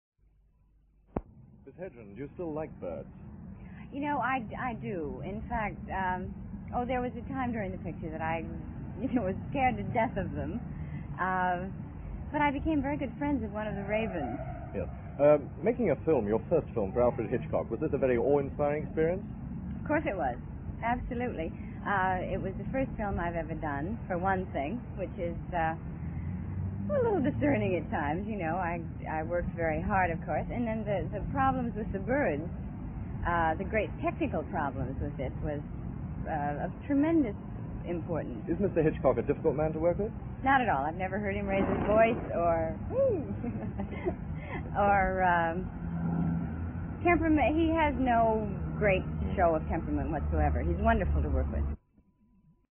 Tippi Hedren 1963
Tippi Hedren interviewed about the film "The Birds".